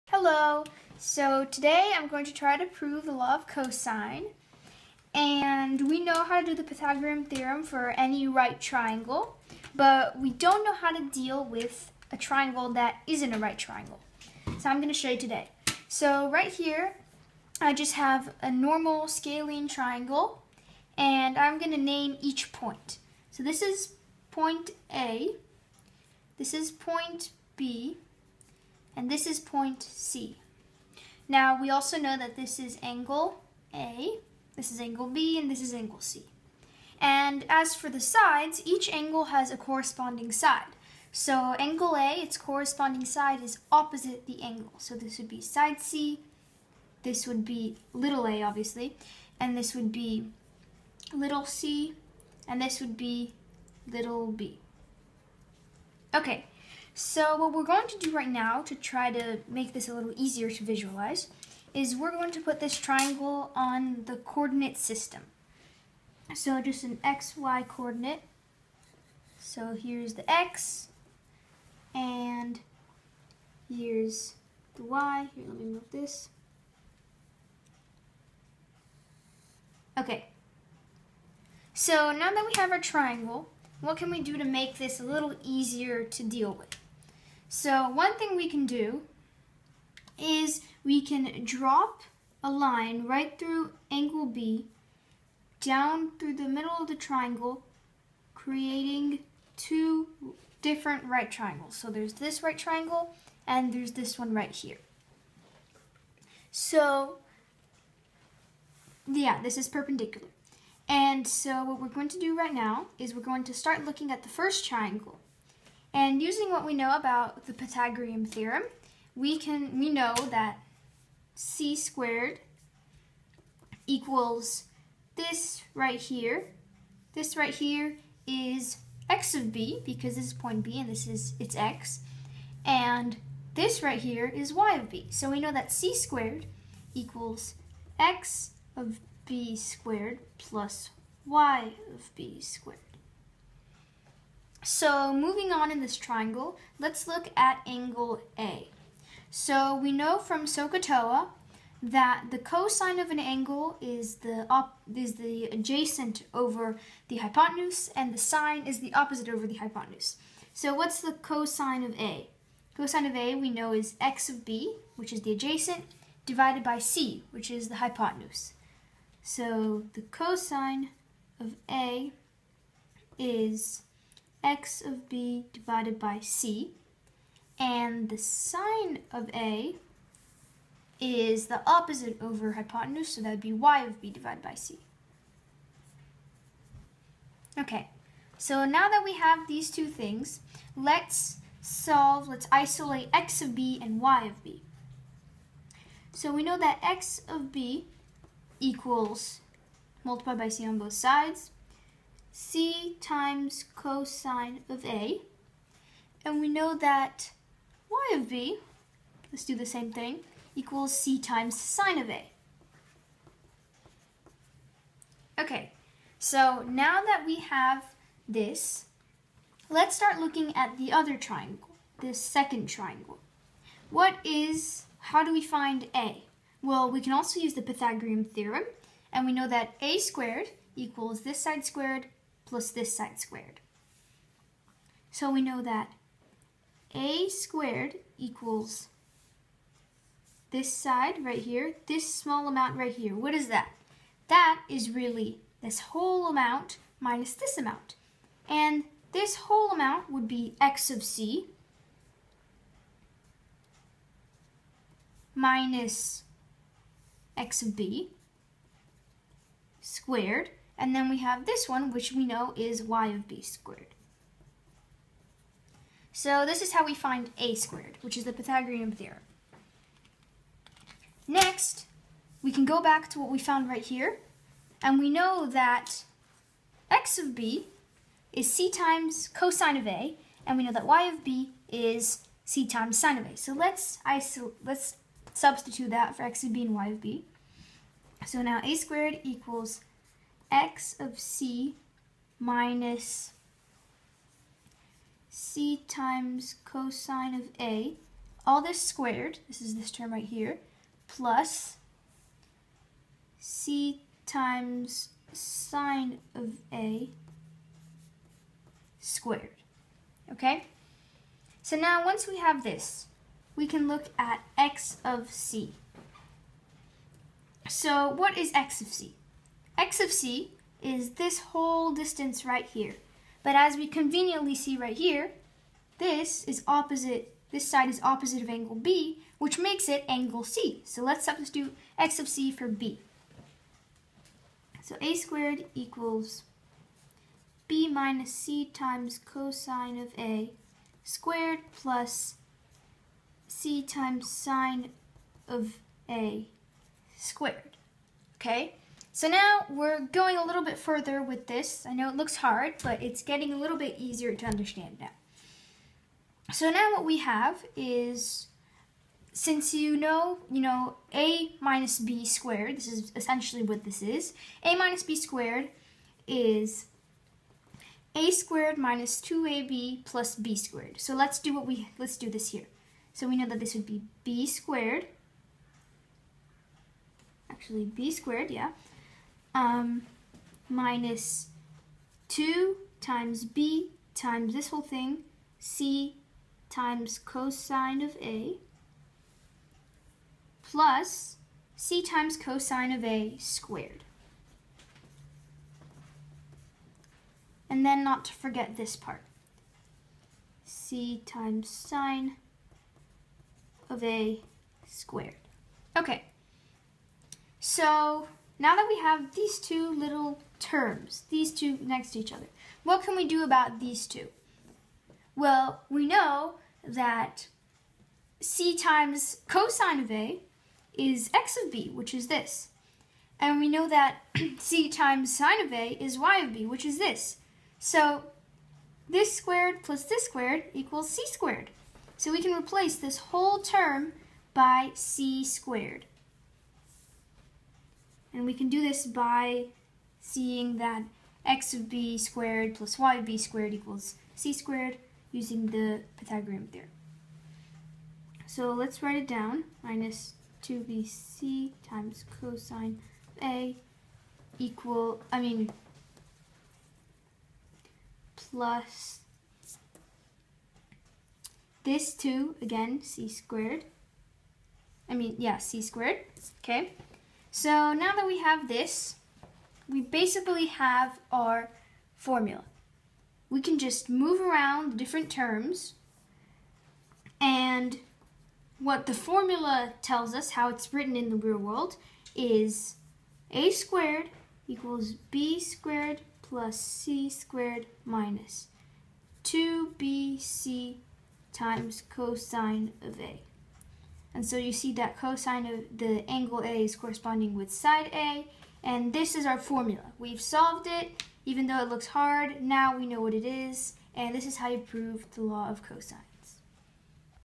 Law of Cosines Proof
13 year old girl shows how to prove the law of cosines with the Pythagorean theorem and a simple coordinate system.

Téa, the 13 year old Salman Khan, teaches and tutors math to middle school students.